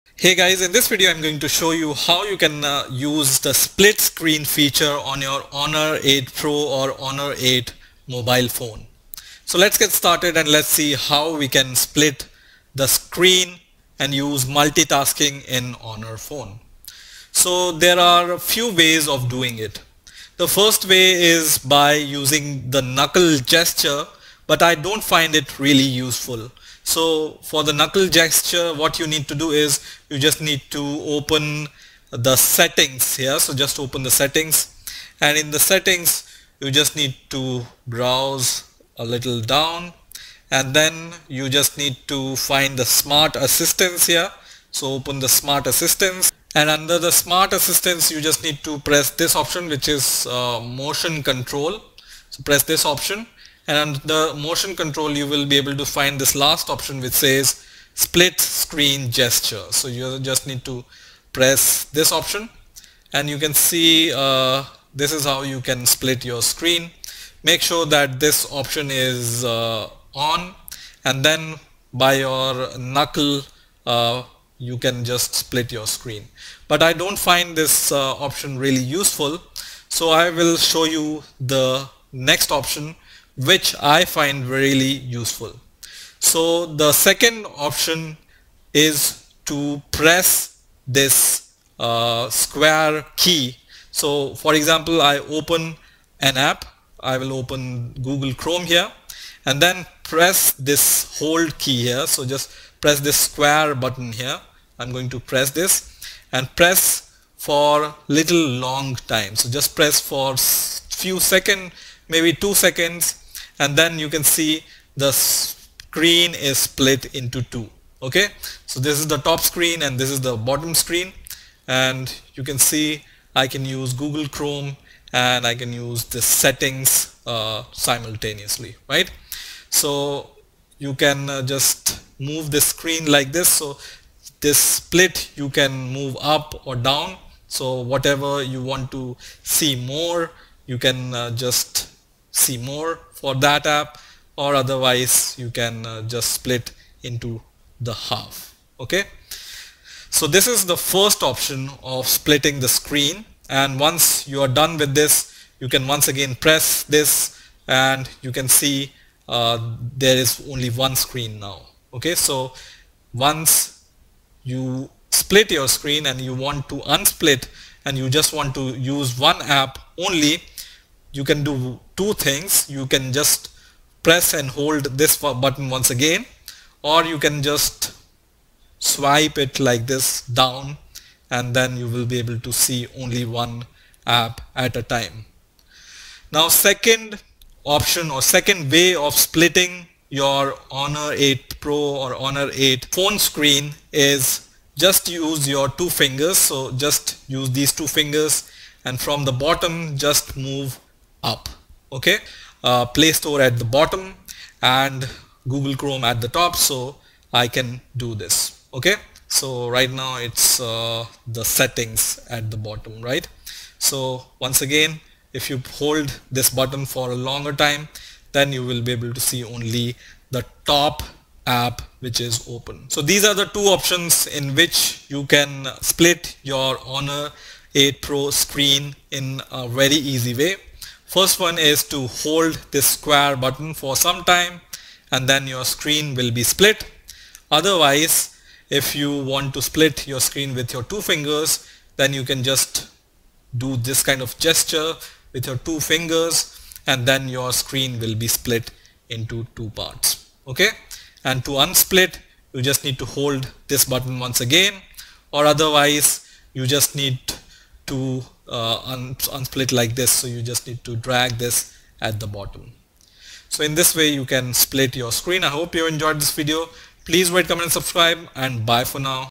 How to Use the Split Screen on HUAWEI Honor 8 Pro (Dual Windows)
Does HUAWEI Honor 8 Pro supports split screens ? the answer is yes, HUAWEI Honor 8 Pro supports Split Screen functionality. Then to activate a split screen, hold the square in the navigation bar (task list button) or draw a line with your knuckle. It’s a Huawei special feature available in all EMUI 5.0 devices